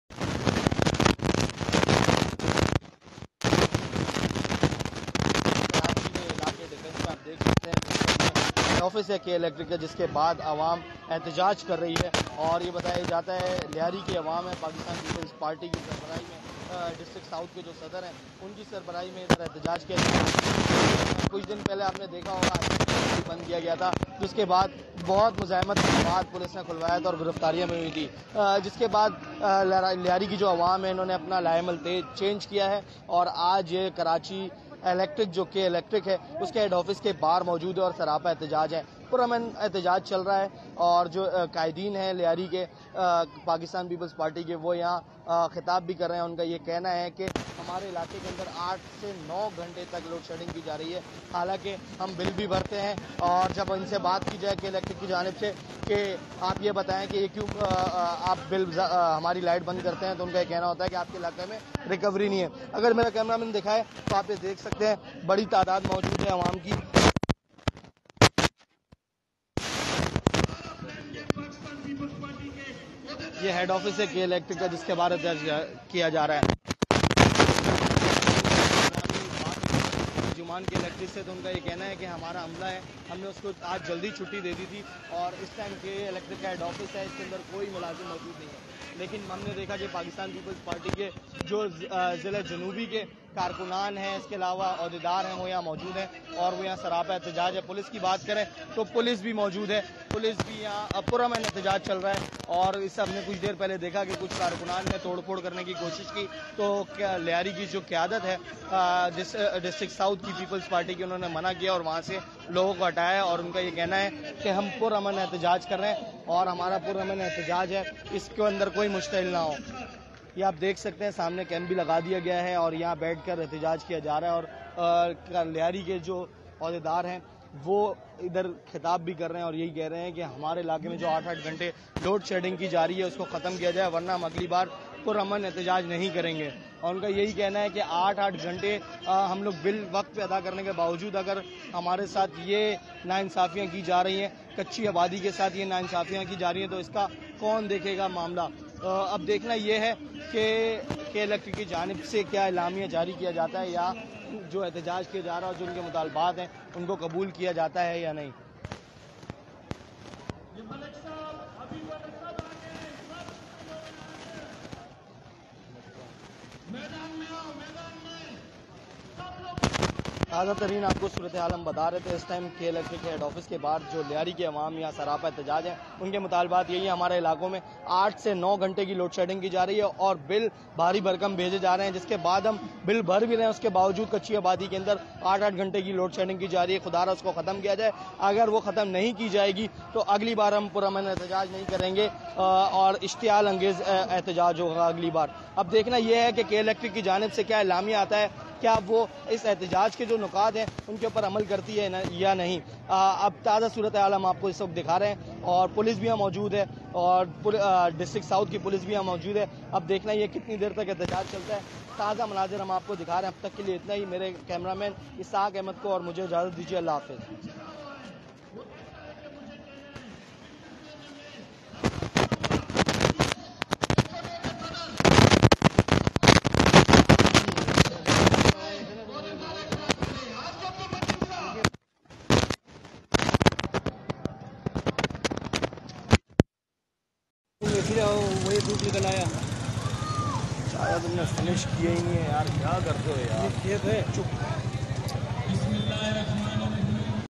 🔴LIVE Exclusive: Public Protest Outside K-Electric Head Office
Karachi: Public protest outside the head office of K-Electric. MM News correspondent Faraz Ahmed is informing you about the latest situation.